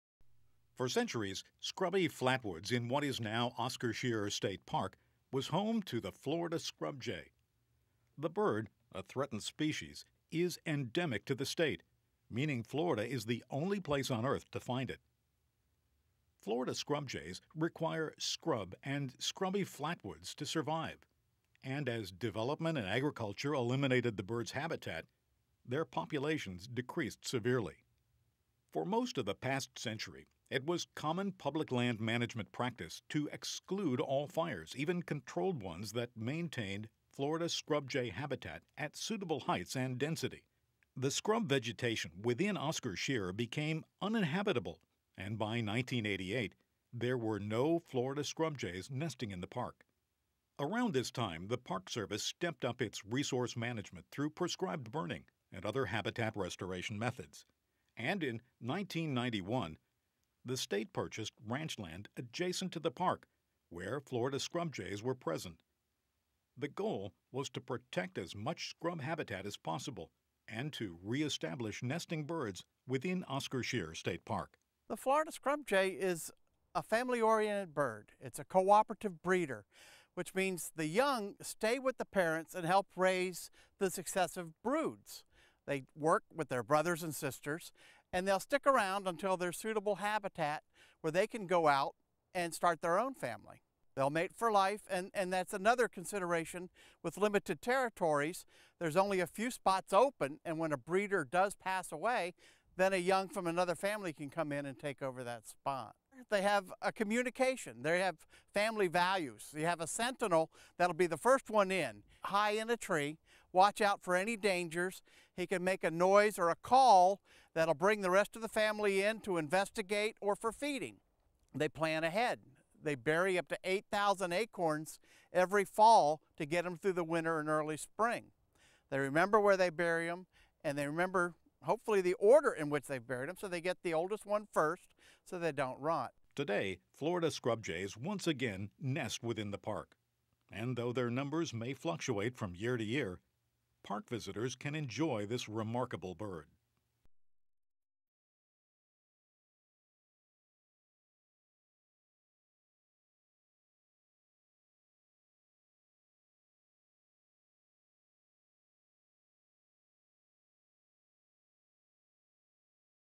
Our Watershed, Our Responsibility: Oscar Scherer State Park, Sarasota, FL
Oscar Scherer State Park: The Florida scrub jay is officially a threatened species. Check out this beautiful and playful bird.